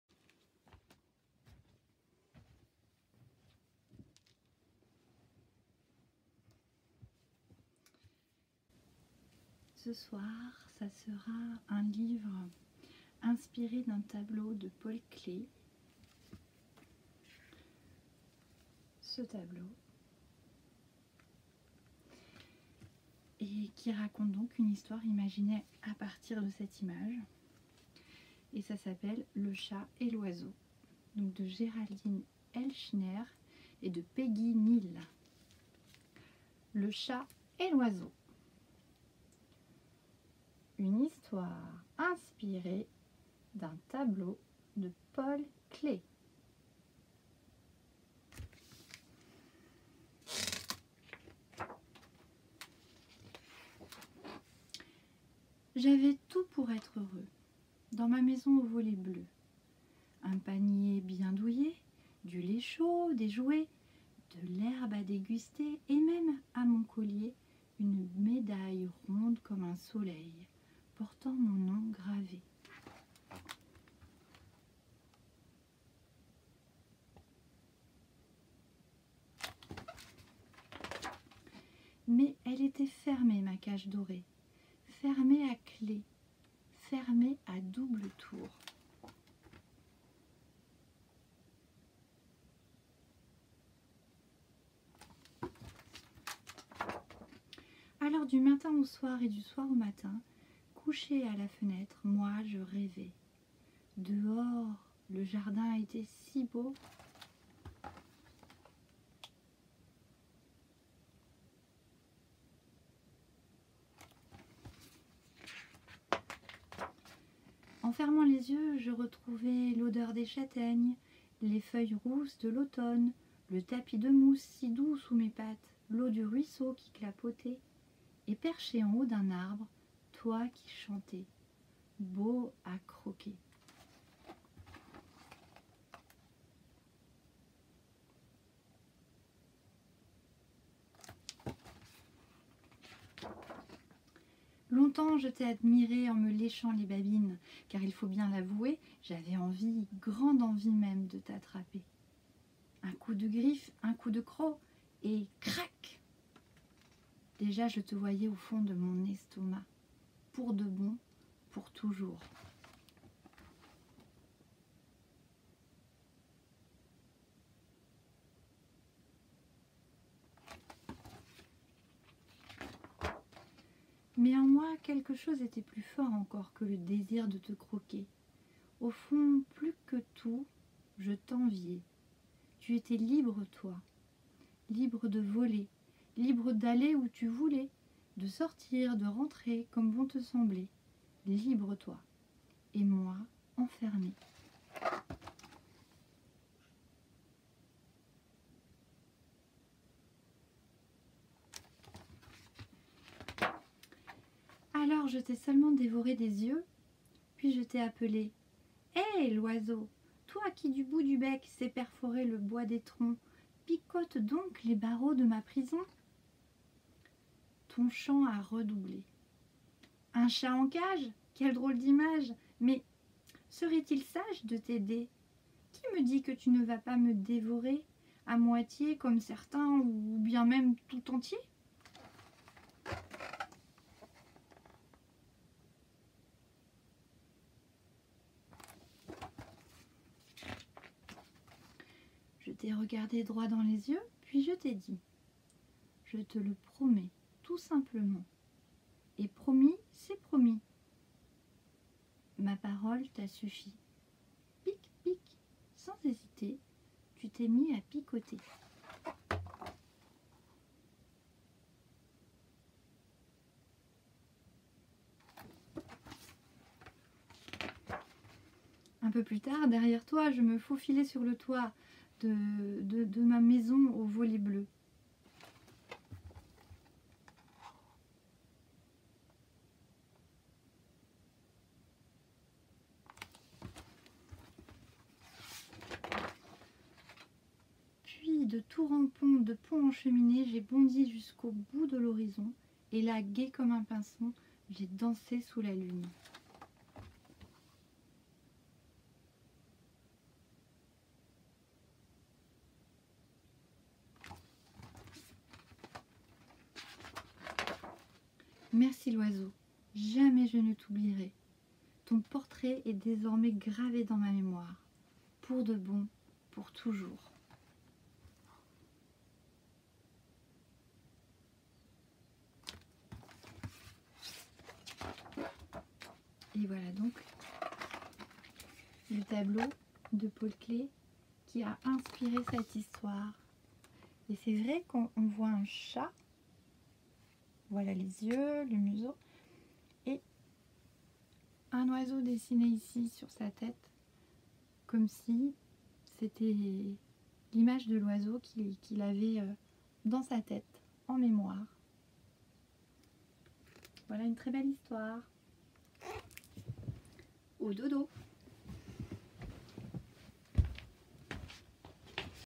Le chat et l'oiseau, lecture
Lecture offerte d'une très belle histoire "le chat et l'oiseau" inspirée d'un tableau de Paul Klee.
Lecture du soir

-continuité pédagogique MS GS-